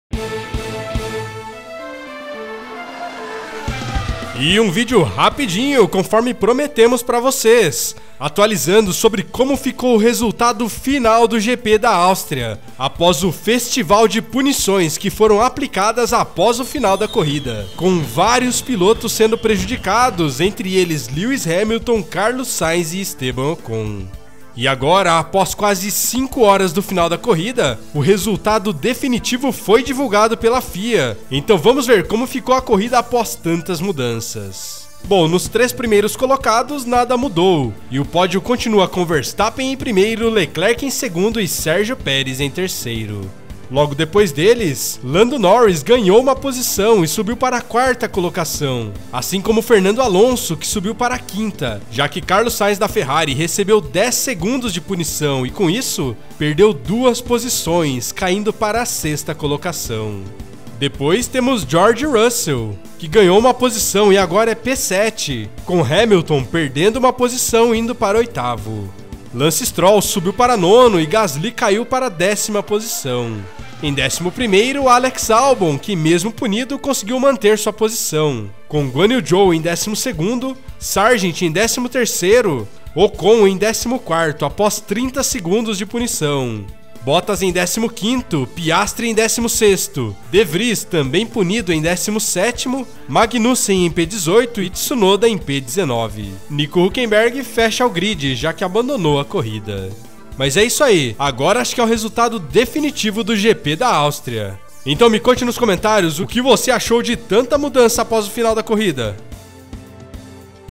URGENTE: VEJA COMO FICOU O GP DA ÁUSTRIA APÓS FESTIVAL DE PUNIÇÕES | FÓRMULA 1 | GP EM CASA
E um vídeo rapidinho, conforme prometemos para vocês, atualizando sobre como ficou o resultado final do GP  da Áustria após o festival de punições que foram aplicadas após o final da corrida, com vários pilotos sendo prejudicados, entre eles Lewis Hamilton, Carlos Sainz e Esteban Ocon.
E agora, após quase cinco horas do final da corrida, o resultado definitivo foi divulgado, então vamos ver como ficou a corrida após tanta mudança. Bom, nos 3 primeiros colocados, nada mudou, e o pódio continua com Verstappen em primeiro, Leclerc em segundo e Perez em terceiro.
Logo depois deles, Lando Norris ganhou uma posição e subiu para a quarta colocação, assim como Fernando Alonso, subindo para a quinta, já que Carlos Sainz recebeu 10 segundos e perdeu 2 posições, caindo para a sexta colocação.
Depois temos Russell, que ganhou uma posição em P7, Hamilton, perdendo uma e indo para oitavo, Stroll subindo para Nono e Gasly caindo uma posição para 10º.
Em décimo primeiro, Albon mesmo punido, conseguiu manter sua posição, com Ghanyu Zhou em 12º, Sargenti em 13º, Ocon em 14ºapós 30 segundos de punição, Bottas em 15º, Piastri em 16º, De Vries, também punido, em 17º, Magnussen em P18 e Tsunoda em P19. Hulkenberg fechou o grid, já que abandonou a corrida.